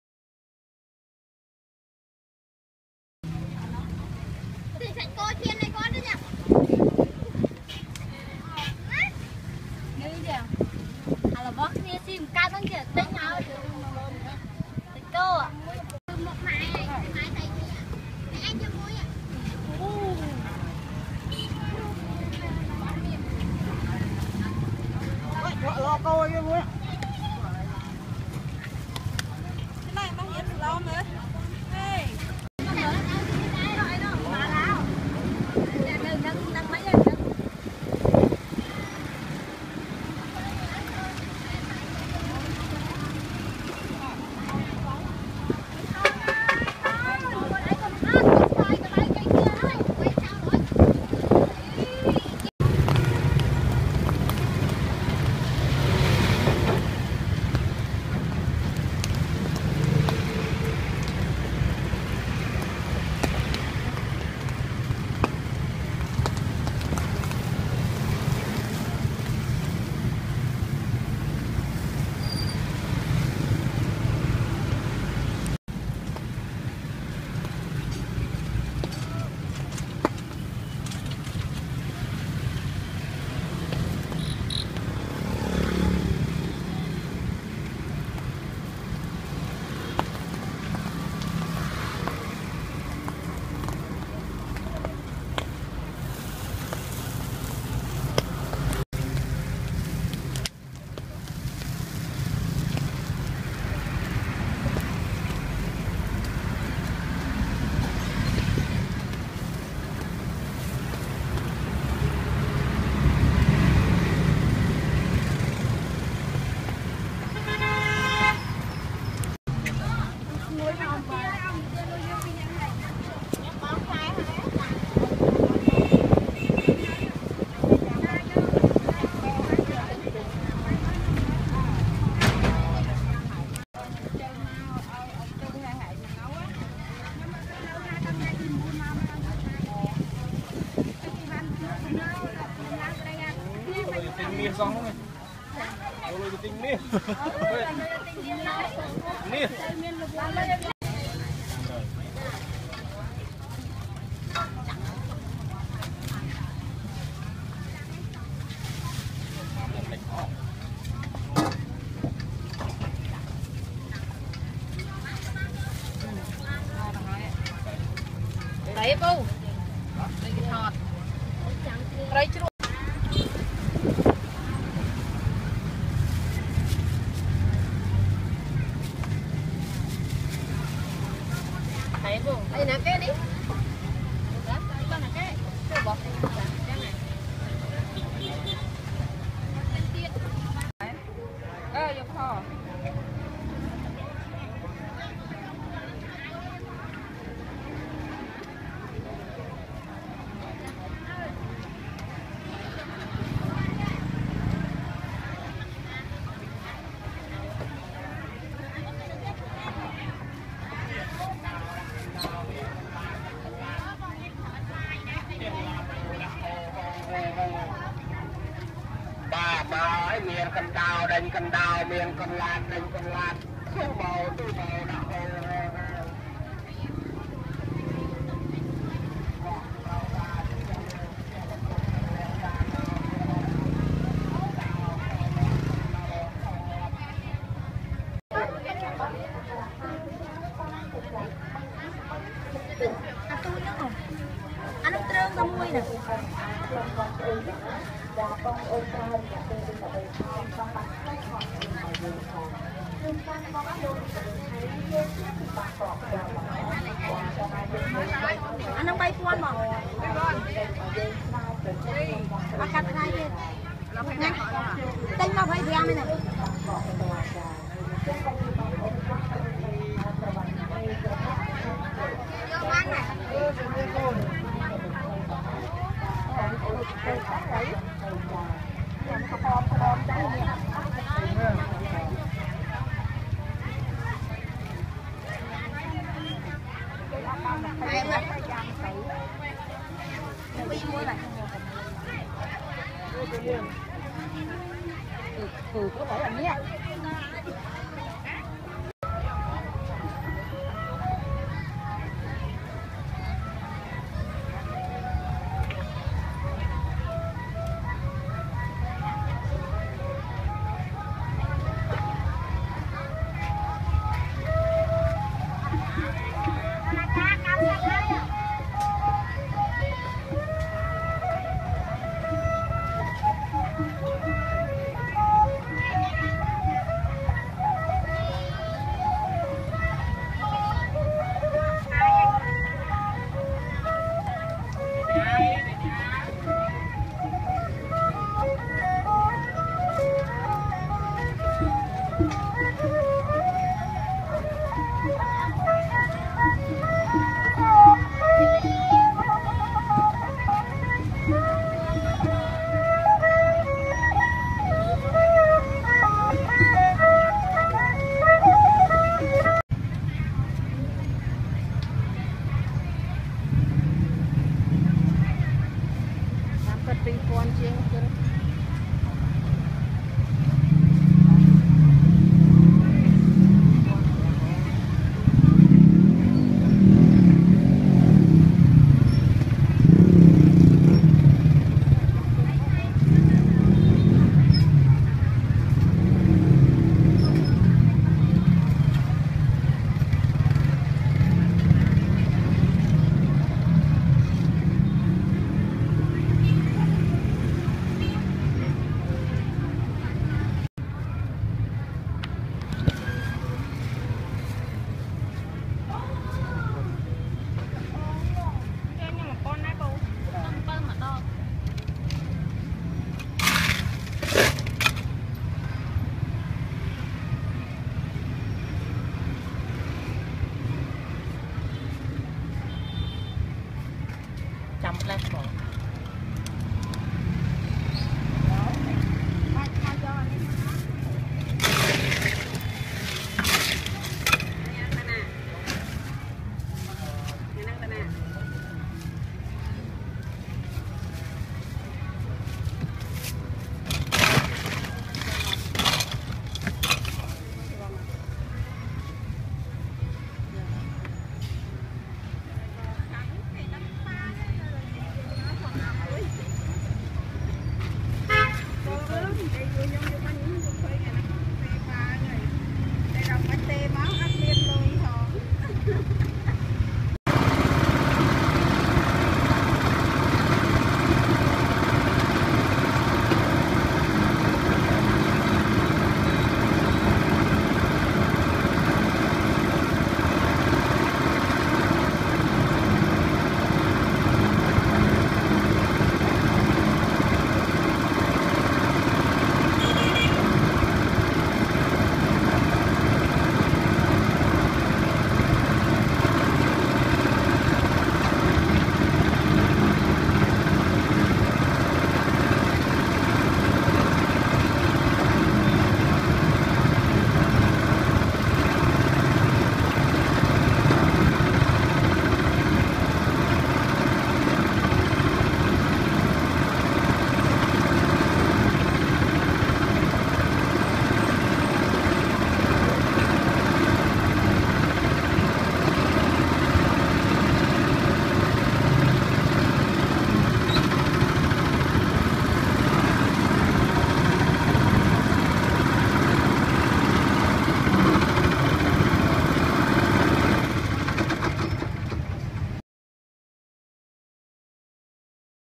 Street Food in Cambodia | Honey Grill Fish with mango sauce | Traditional Asia Cheese
Street Food in Cambodia | Honey Grill Fish with mango sauce | Traditional Asia Cheese | Palm Fruit | Taro and Tomato Natural Fruit